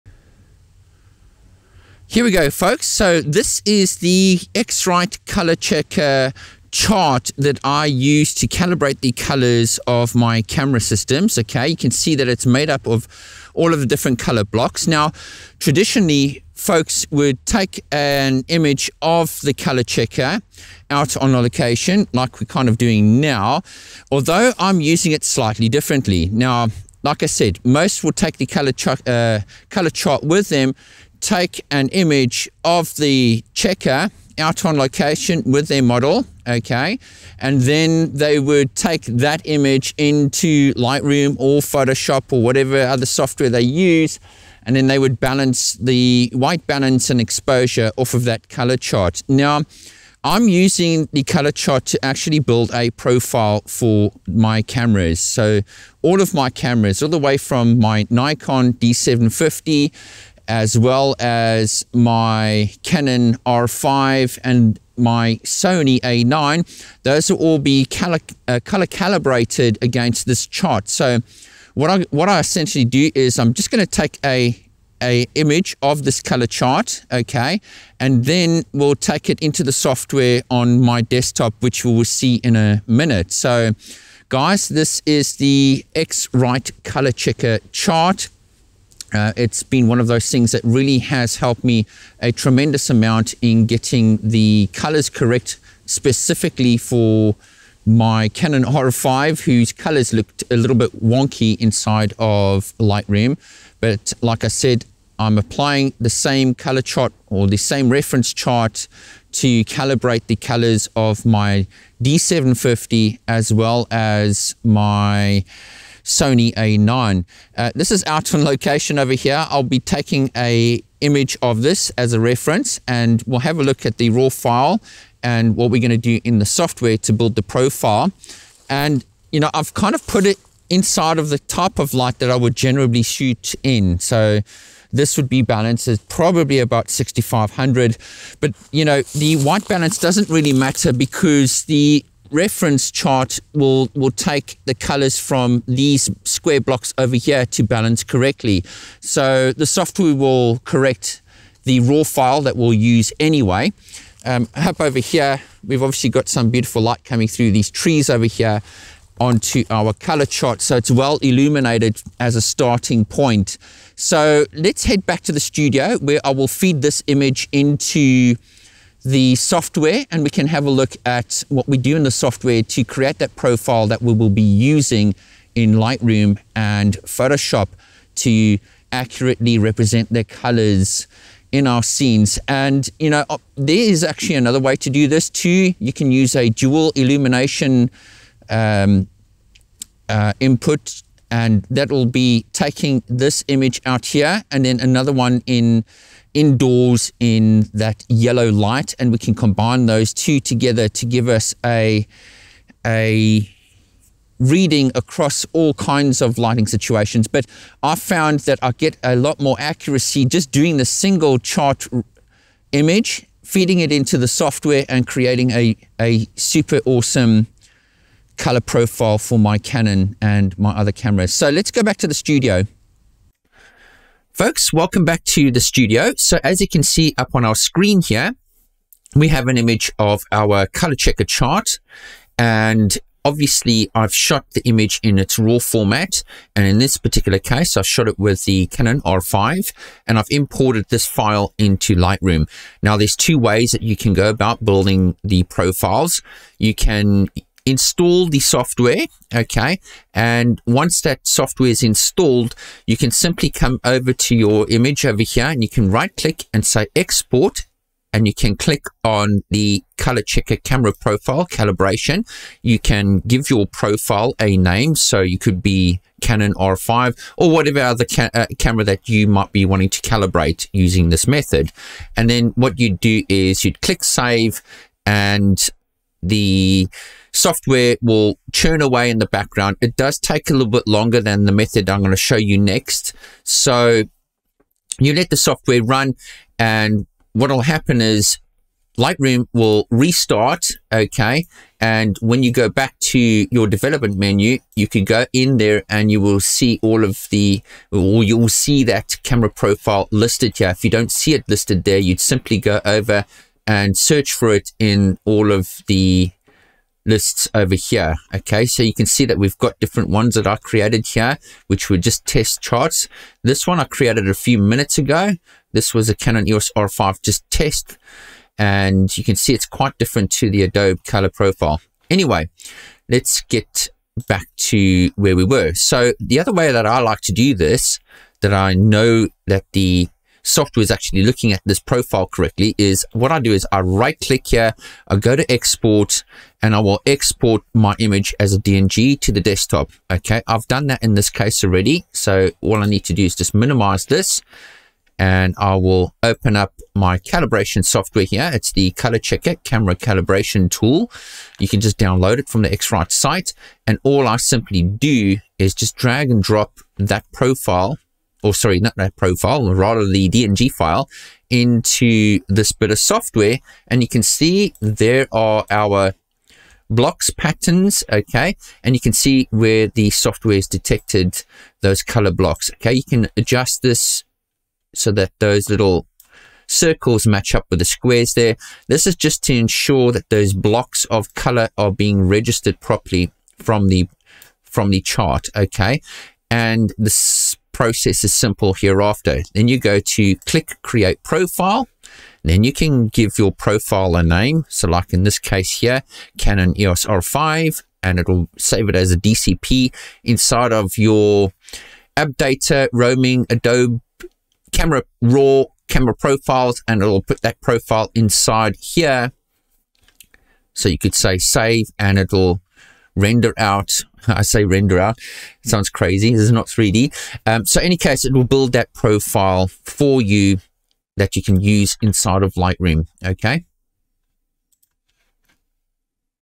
My Colorchecker process
In this video, I'll show you how I use the X-rite ColorChecker Passport Photo 2 chart and software to build a camera profile for accurate colors